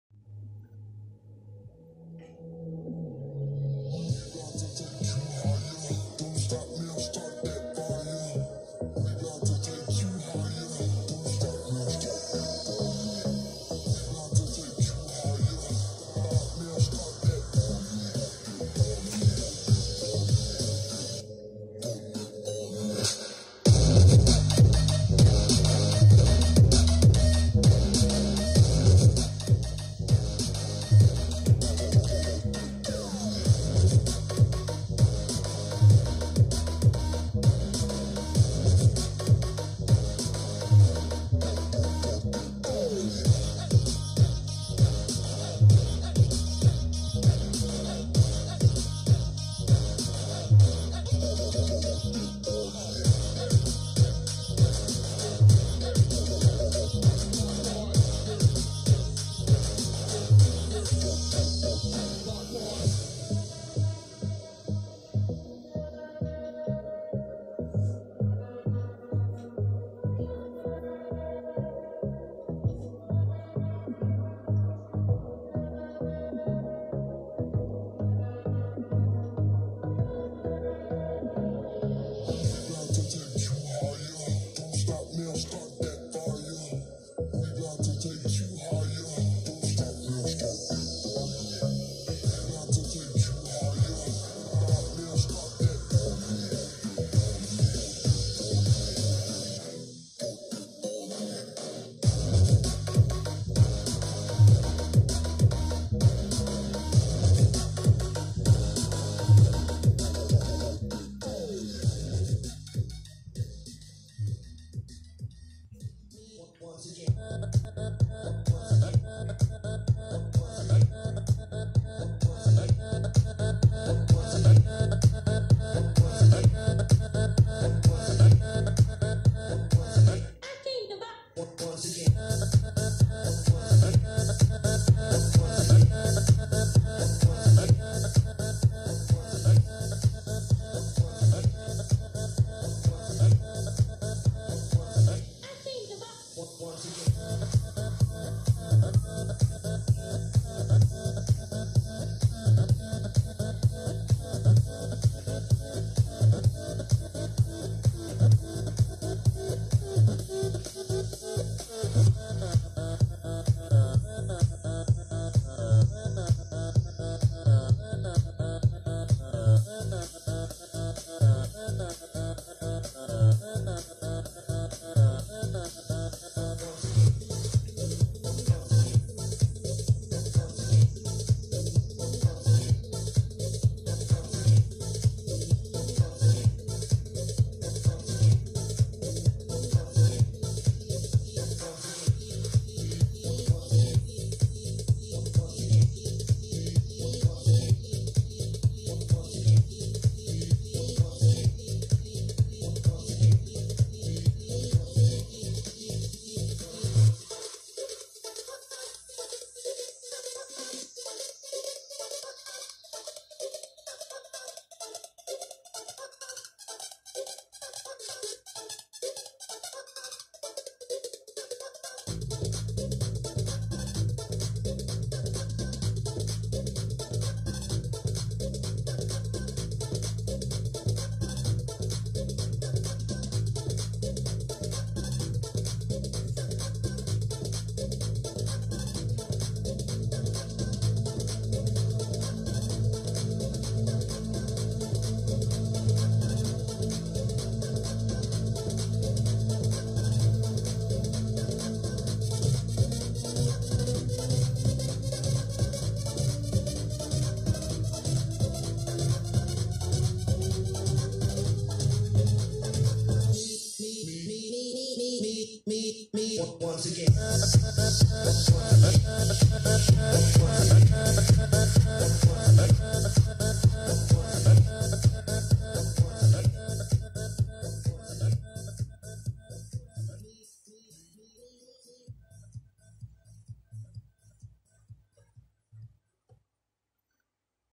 woxter big bass 110/mis nuevos altavoces
estos son mis nuevos altavoces , suenan muy fuertes si apoyais el video subo otro video poniéndolo al 100% de volumen ya que en este video esta al 40%